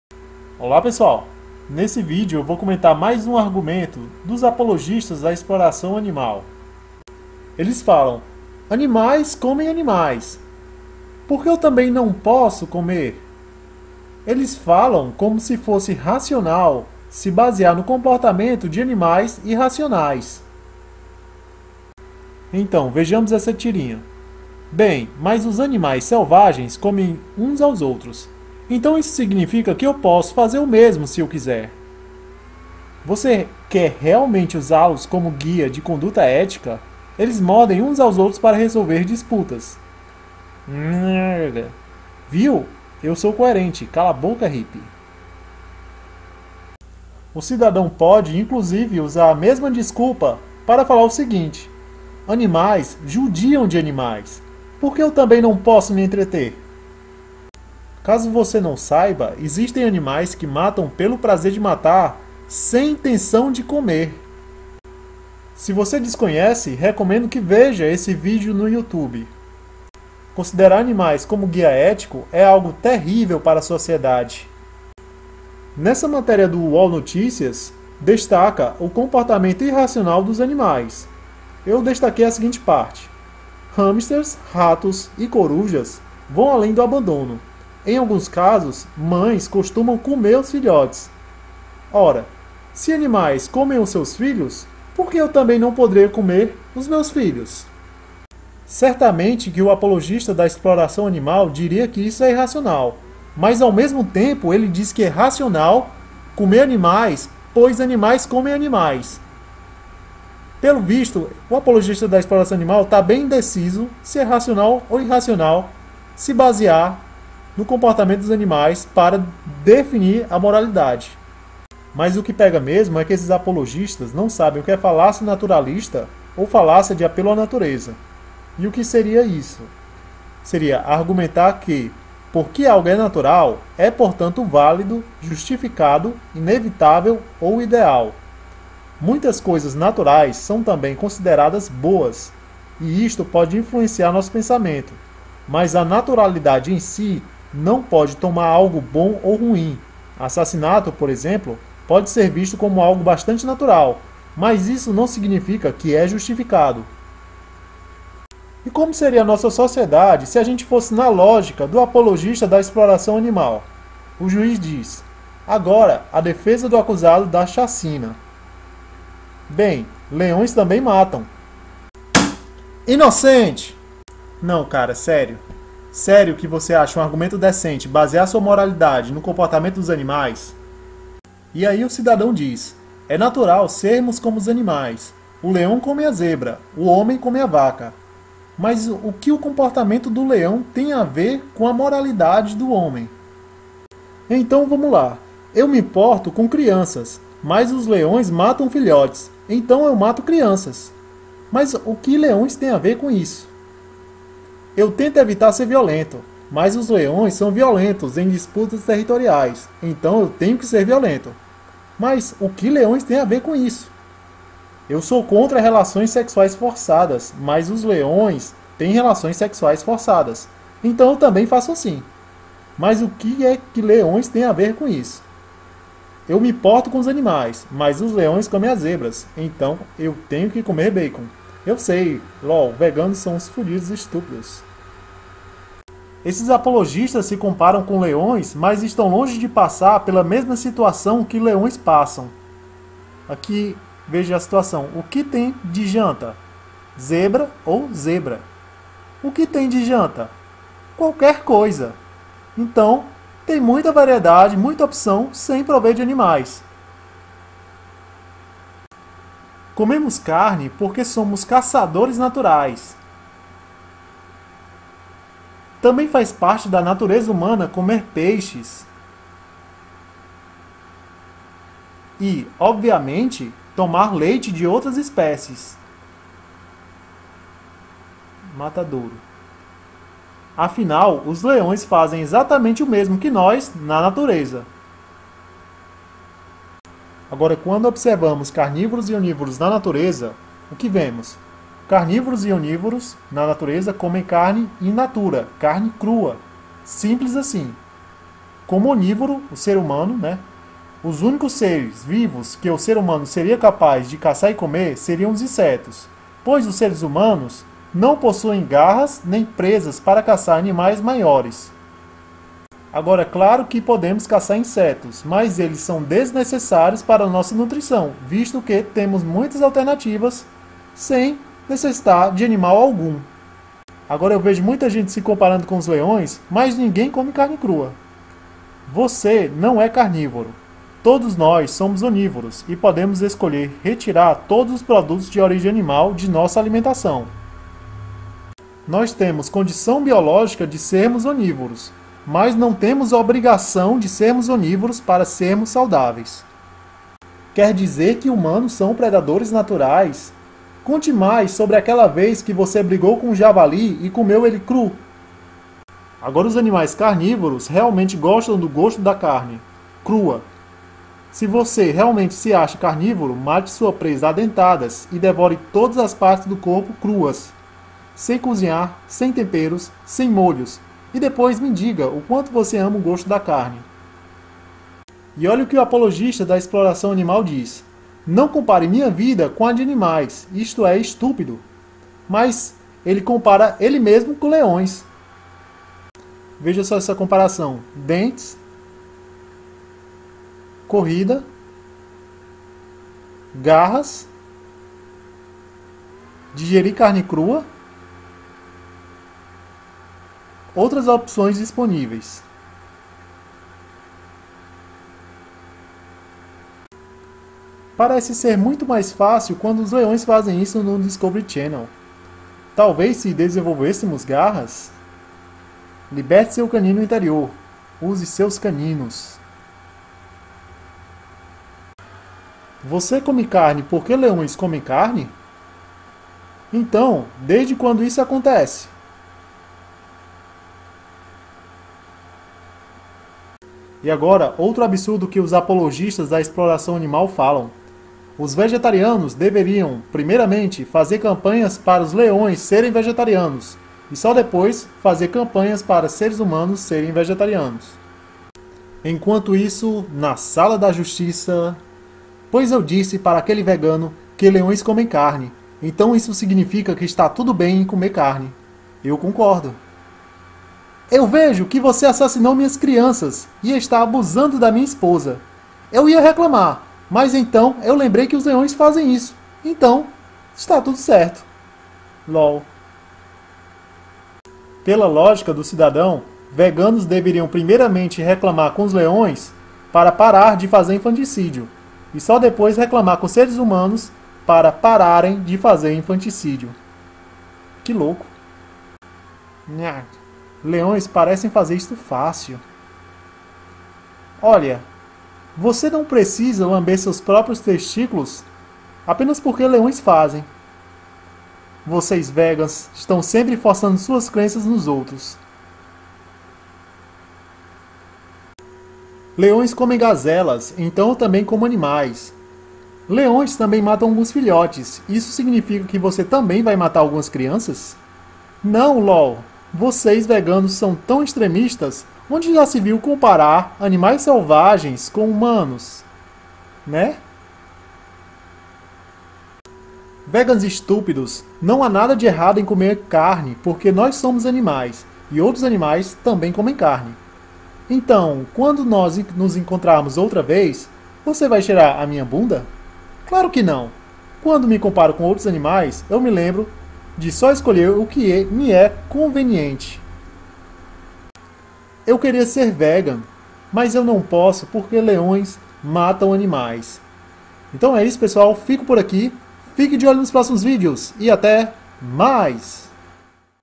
Se animais comem animais, seria motivo para nós comermos?
É... sempre aparece aquele papo de que se um animal faz X, então não tem nada de errado em fazer X tambem.

A ética é uma construção artificial humana, não da natureza. Então não cabe nem citar a natureza quando falamos de ética. Do contrario formaria-se falacia naturalista.

A Ética leva em conta os interesses que não são seus.

Te deu vontade de comer um animal cru?